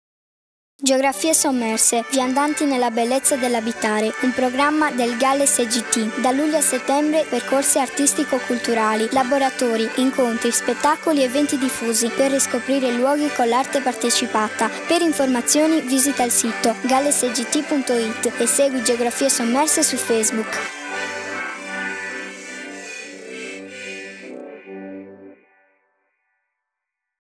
Geografie Sommerse - GAL SGT
Geografie Sommerse. Viandanti nella bellezza dell'abitare il territorio del GAL SGT" è: spettacoli, installazioni, itinerari sensoriali e agroalimentari, laboratori ed eventi diffusi nel territorio del Sarrabus, Gerrei, Trexenta e Campidano di Cagliari. Da giugno a settembre 2015. Info su e